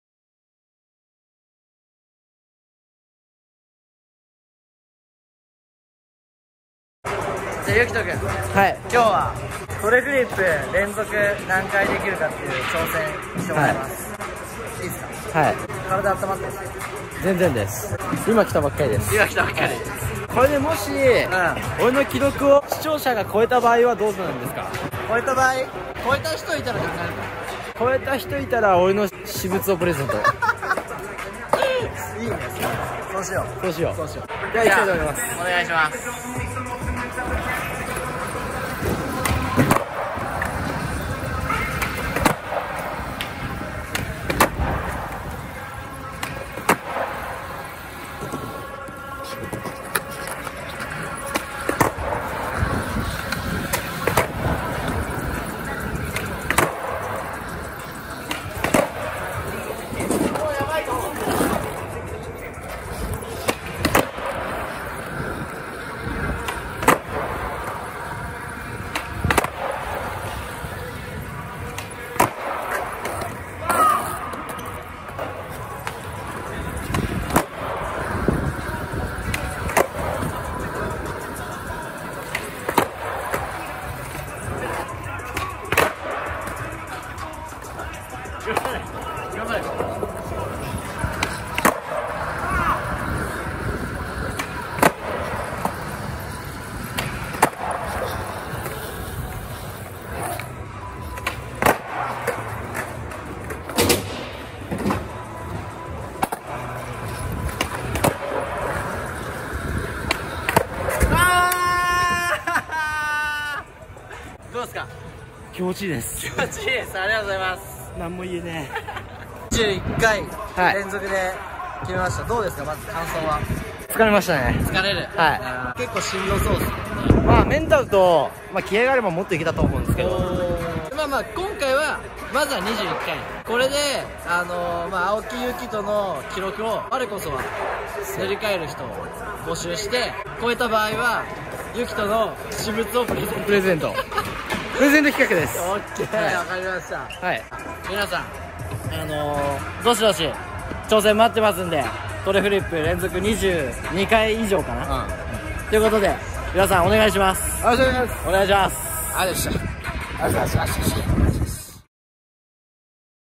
青木ユキトによる！トレフリップ連続何回できるかチャレンジ！
＊プレゼント企画は終了しています

↓スケボー関連の個人的に良く見ているオススメ動画です